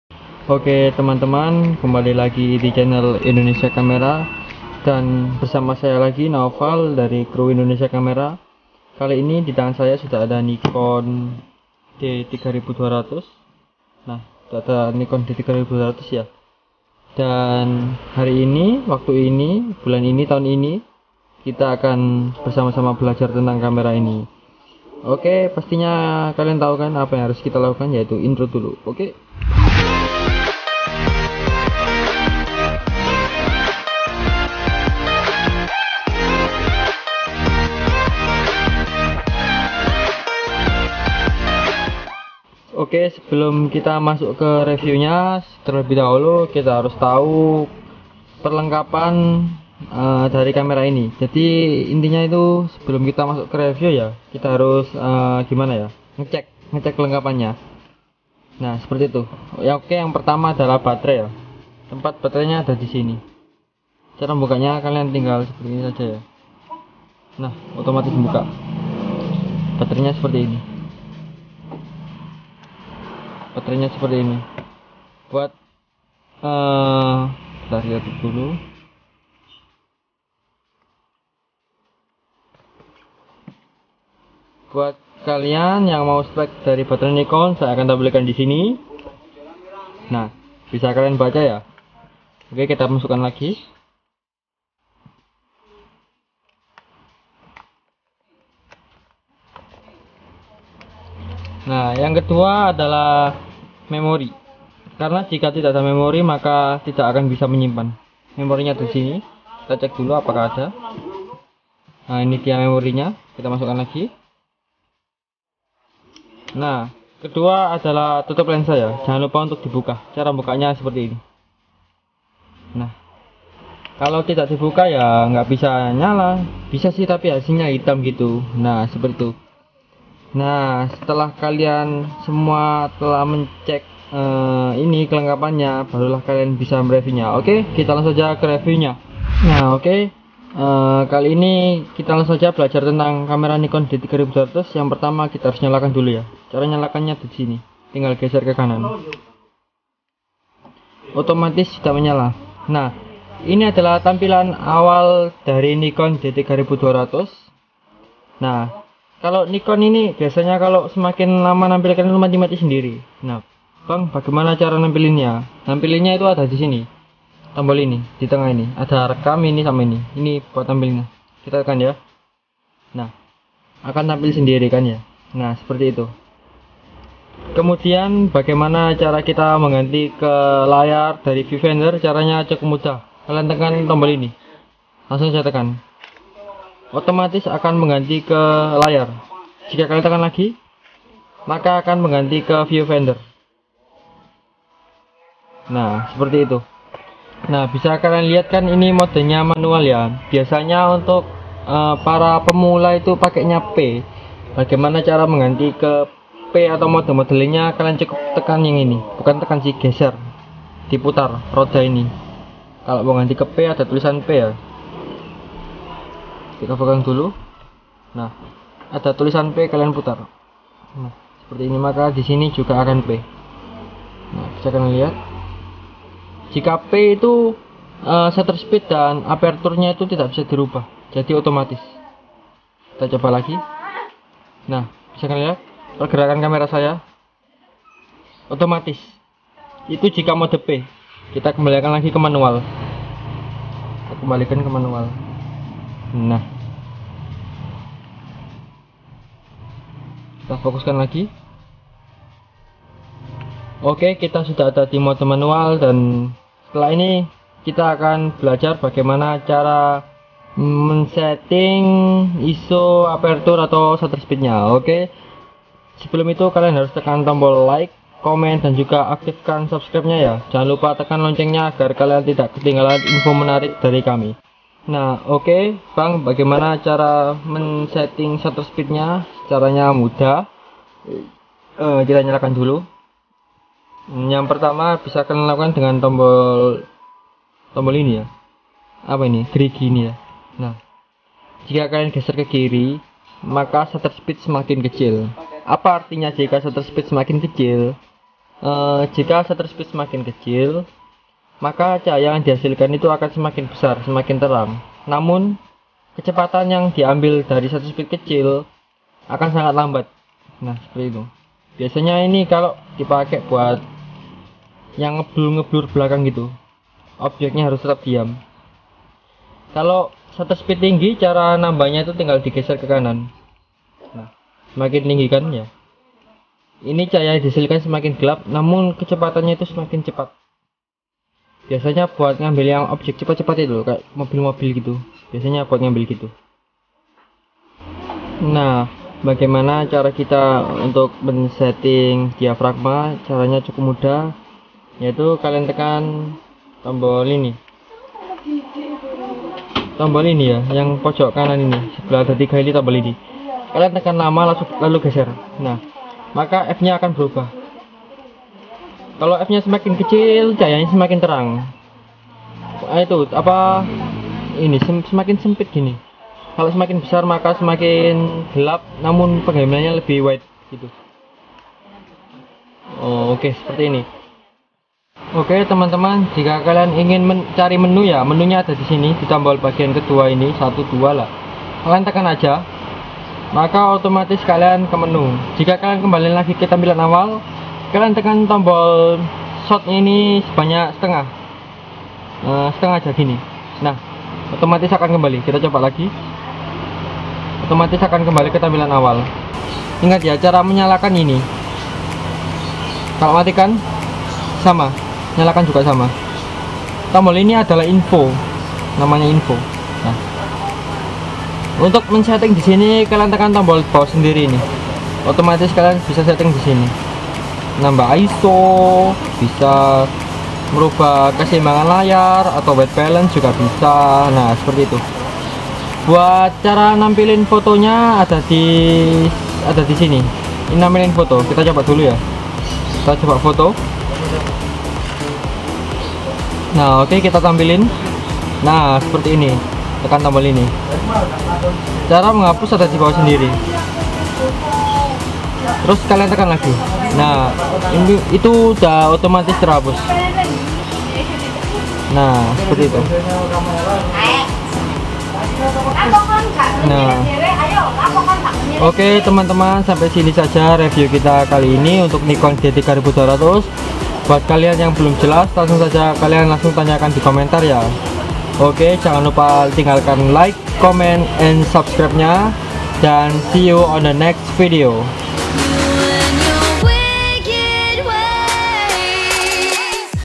Tutorial Nikon D3200 | How to take great photos like a pro!
Hai sahabat Indonesia Kamera
Kali ini kami akan mereview camera Dslr Nikon D3200 dari kami
jangan lupa

BIla kurang jelas langsung saja tulis di kolom komentar